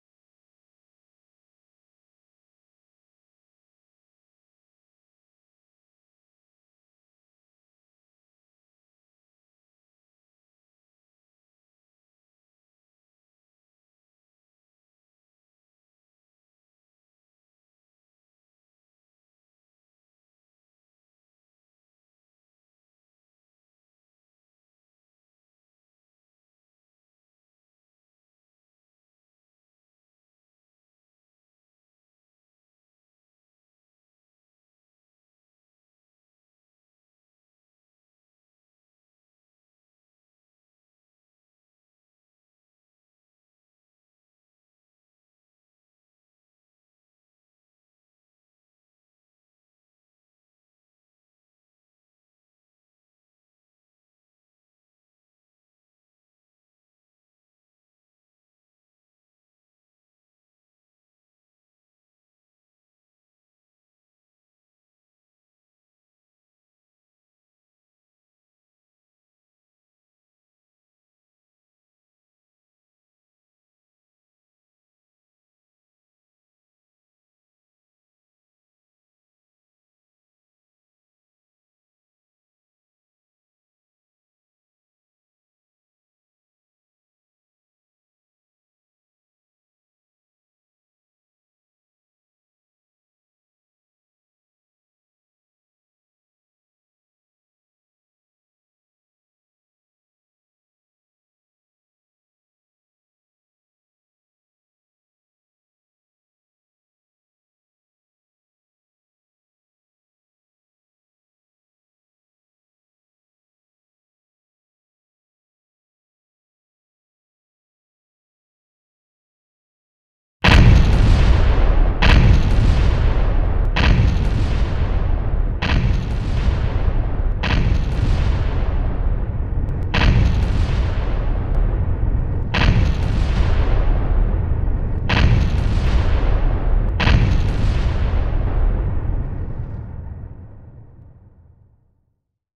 2 Minute Timer Bomb Egg 💣🥚
2 Minute Timer Bomb Egg 💣🥚
At the end animation explosion.